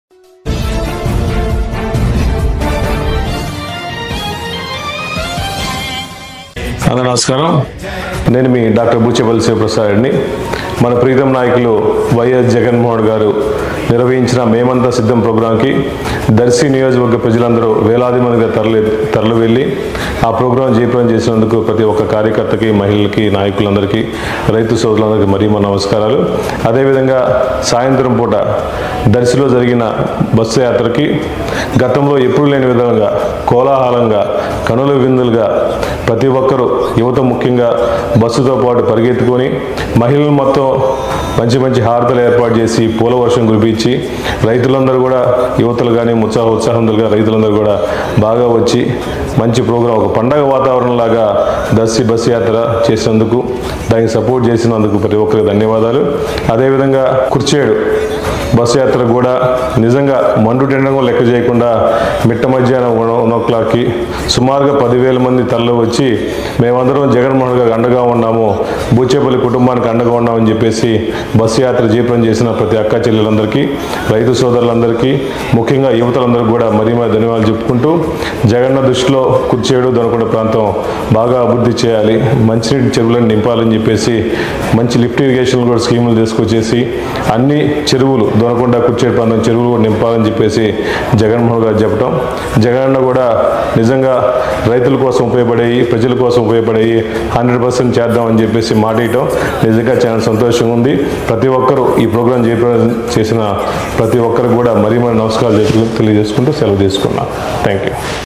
దర్శిలో జగనన్న బస్సు యాత్ర జయప్రదం చేసిన ప్రతిఒక్కరికి ధన్యవాదాలు.. బూచేపల్లి శివప్రసాద్ రెడ్డి
జగనన్న బస్సు యాత్ర జయప్రదం చేసిన ప్రతి ఒక్కరికి ధన్యవాదాలు.. బూచేపల్లి శివప్రసాద్ రెడ్డి